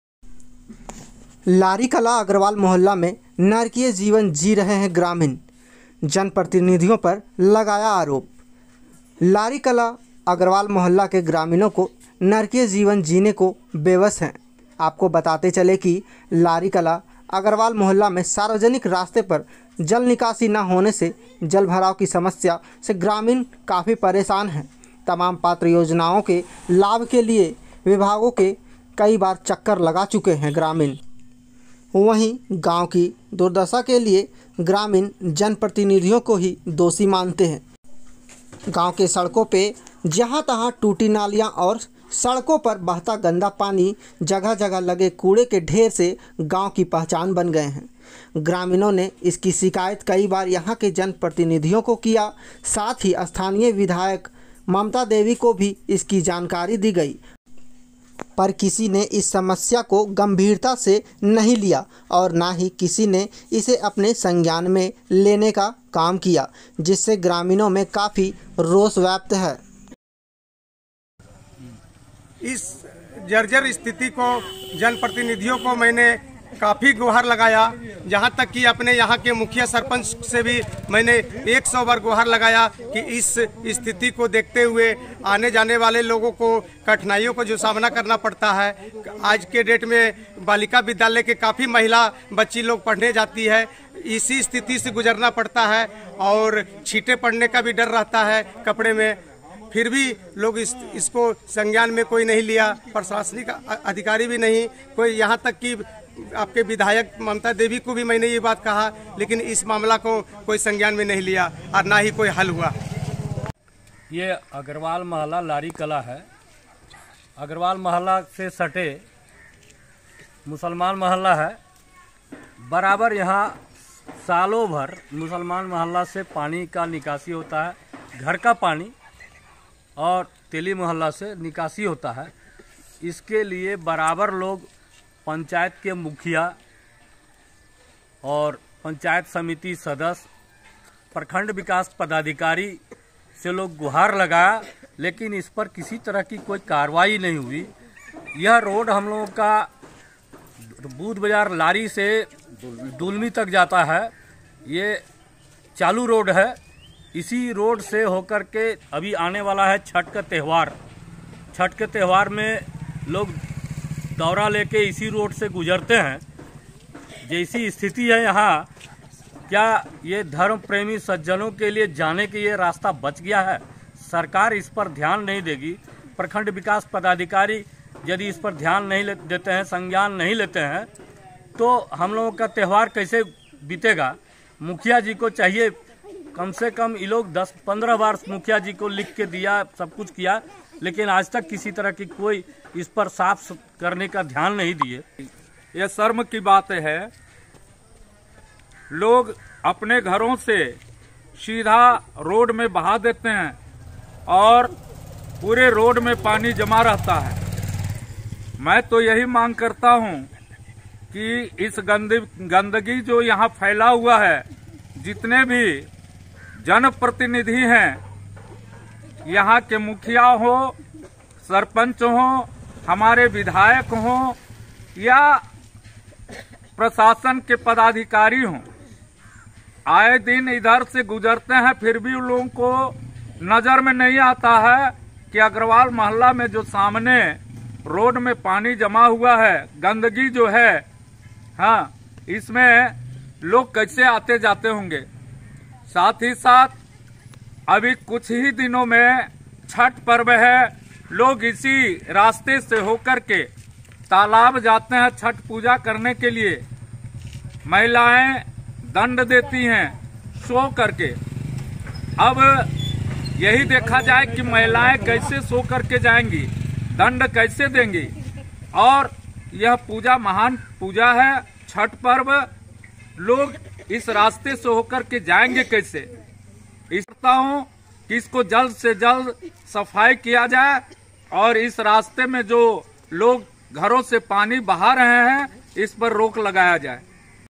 लारिकला अग्रवाल मुहल्ला में नारकीय जीवन जीने को मज़बूर हैं ग्रामीण// जनप्रतिनिधियों पर लगाया आरोप
लारिकला अग्रवाल मुहल्ला में नारकीय जीवन जीने को हसि मजबूर ग्रामीण, लोगो ने जनप्रतिनिधियों पर लगाया आरोप

लारिकला अग्रवाल मुहल्ला के ग्रामीणों को नारकीय जीवन जीने को है बेबस बताते चले के लारिकला अग्रवाल मुहल्ला में सार्वजनिक रास्ते पर जलनिकासी न होने से जलभराव की समस्या से ग्रामीण परेशान हैं। तमाम पात्र योजनाओं के लाभ के लिए विभागों के चक्कर लगा रहे हैं ग्रामीण गांव की दुर्दशा के लिए ग्रामीण जनप्रतिनिधियों को दोषी मानते हैं।
वंही गांव में अव्यवस्थाएं हीं नजर आती है्। उखड़े खड़ंजे, टूटी नालियां और सड़कों पर बहता गंदा पानी, जगह जगह लगे कूड़े के ढेर गांव की पहचान बन गए हैं। ग्रामीणों ने इसकी शिकायत कई बार यंहा के जनप्रतिनिधियों को किया साथ ही स्थानीय विधायक को भी इसकी जानकारी दी गयी पर किसी ने इस समस्या को गम्भीरता से नही लिया और न ही किसी ने इसे अपने संज्ञान में लेने का काम किया। जिससे ग्रामीणों में काफी रोष व्याप्त है।